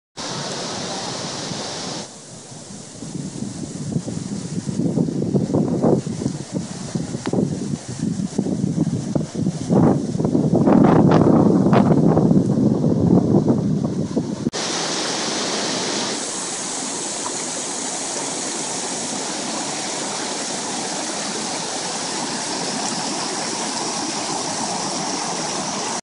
This is the day after...
Waters went over our walk bridge, creek still over our causeway.